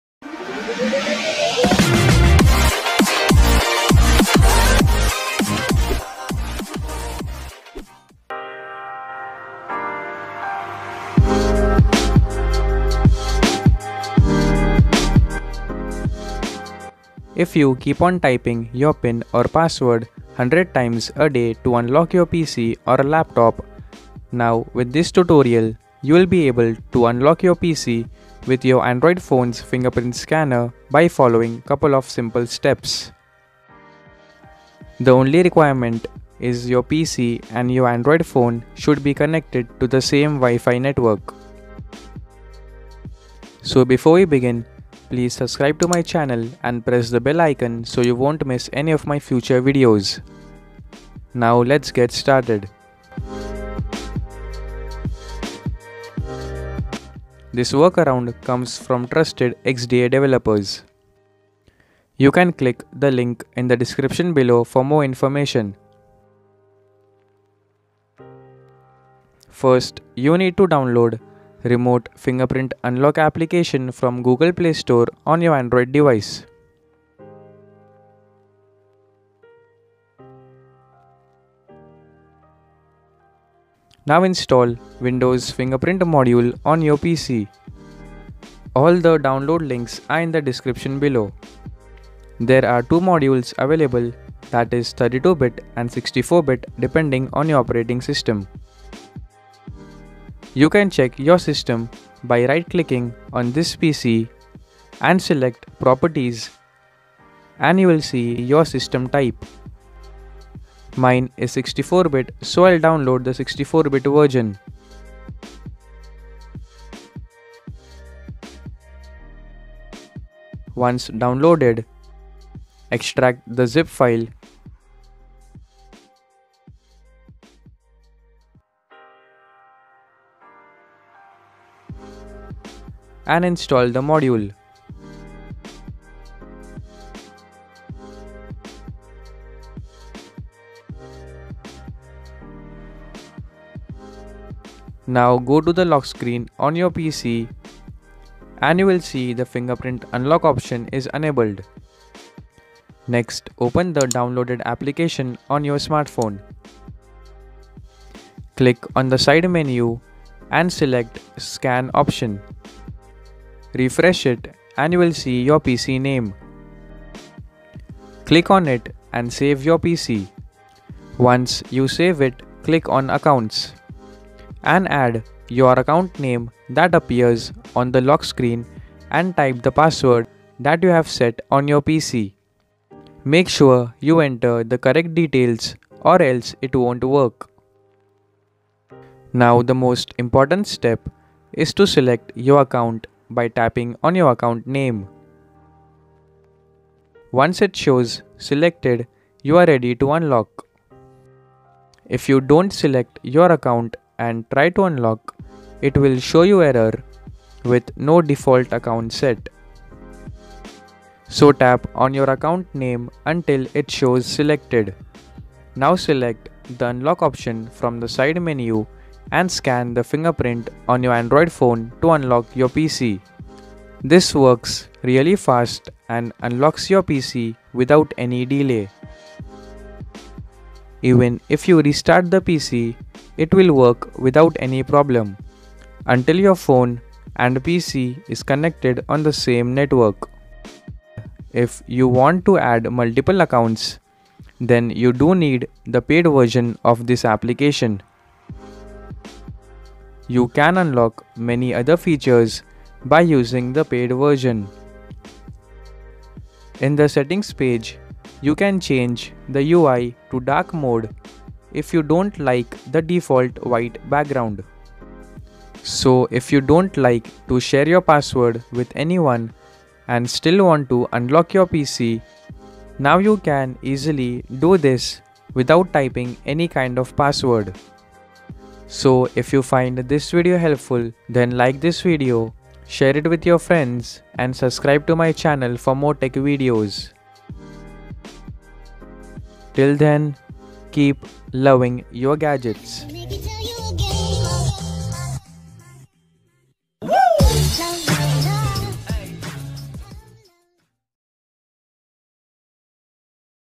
Unlock PC With Android Fingerprint Scanner : Simple Steps
In this video we take a look on a tutorial for unlocking your Windows PC or Laptop by using your Android Smartphone fingerprint scanner.
This is a small procedure and works fine when you follow the steps shown in the video.
Thanks to XDA developers for making this workaround for users to unlock their Windows PC using their Android Smartphone finger print scanner.

This video contains information on
How to unlock PC with Android Fingerprint
Unlock PC using android fingerprint scanner

Keep Loving Your Gadgets